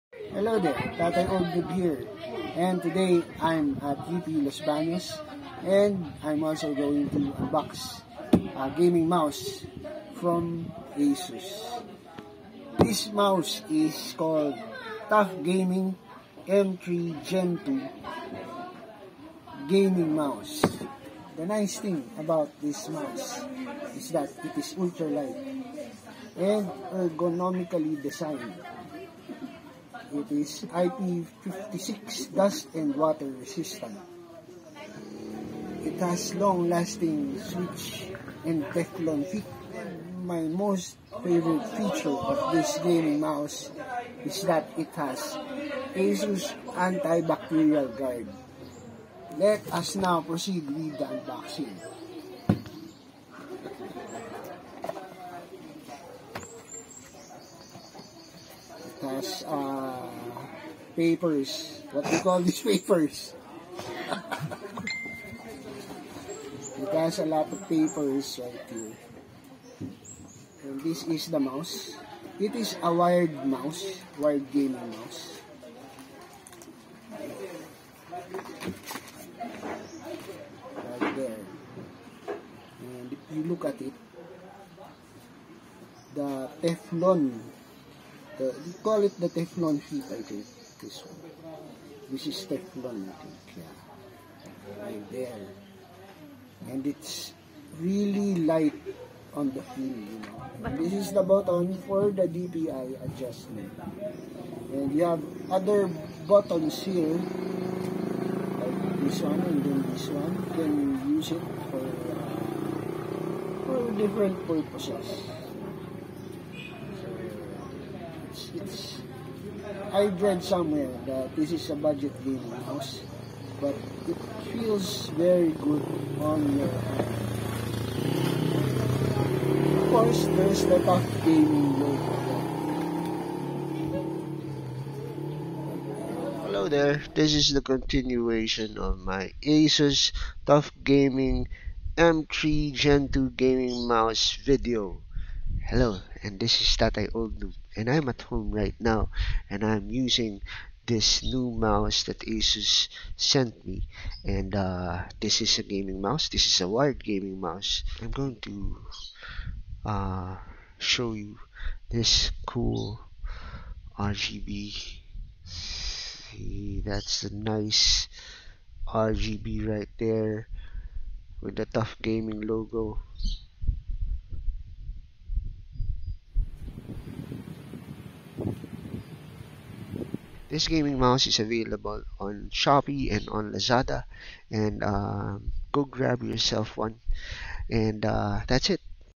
Asus TUF Gaming M3 Gen II Gaming Mouse
Unboxing video of Asus TUF Gaming M3 Gen II Gaming Mouse.